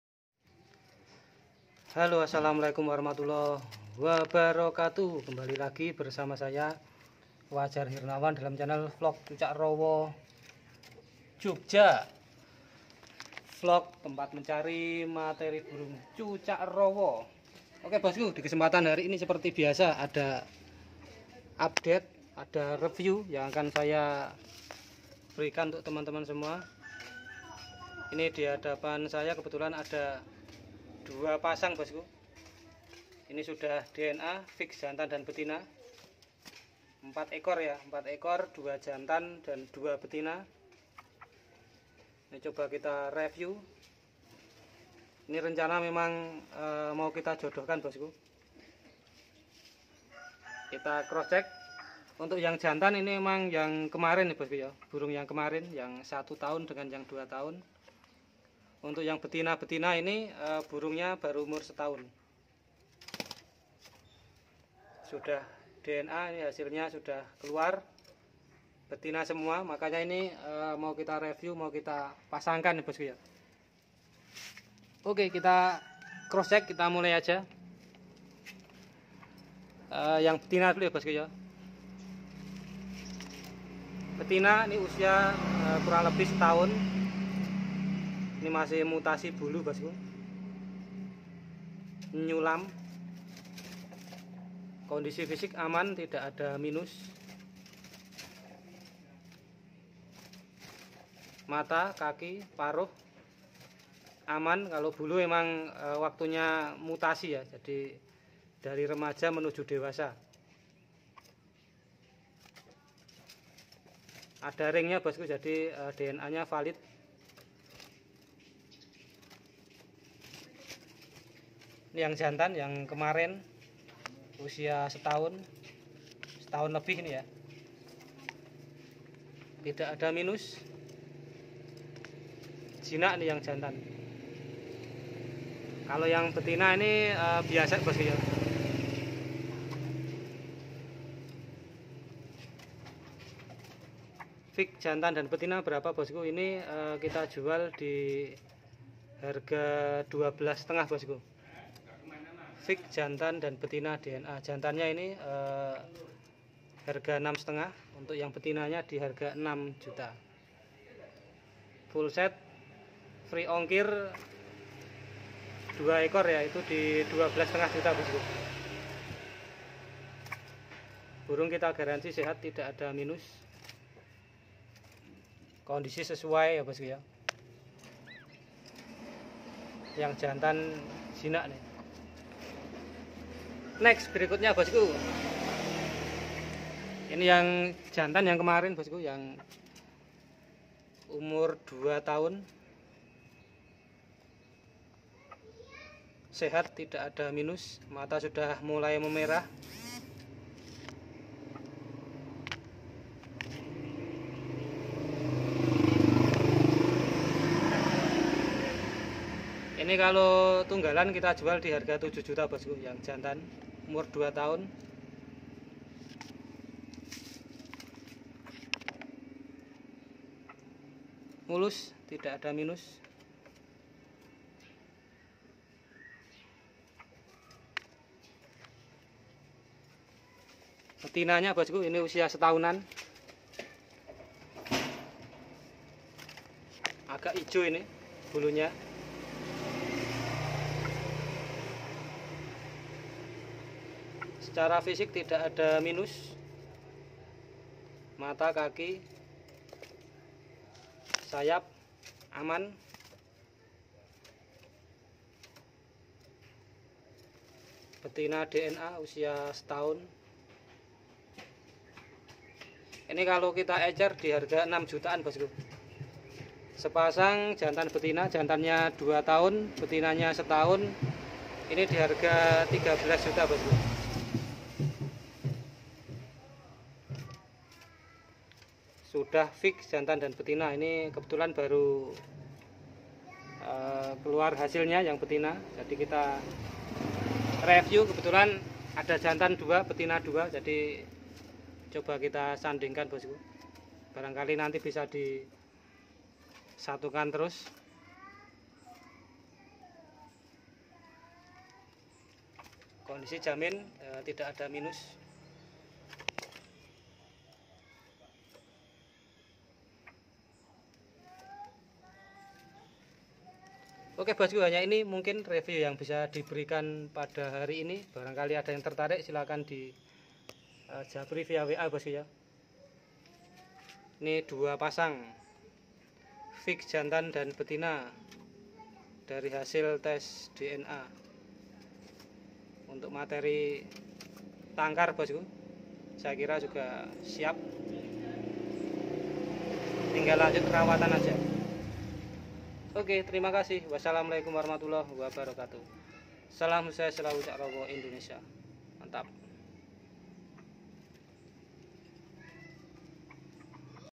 Cucak Rowo 1 Tahunan Fix Jantan Betina DNA 12,5 juta.. WA 085101678509
Cucak Rowo Sudah tes DNA
Jantan 1 tahunan 6,5 juta
Betina kisaran 1 tahun 6 juta
Jantan 2 tahun 7 juta
Betina 1 tahun 6 juta
Burung sehat tidak ada minus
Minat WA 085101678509
W Hernawan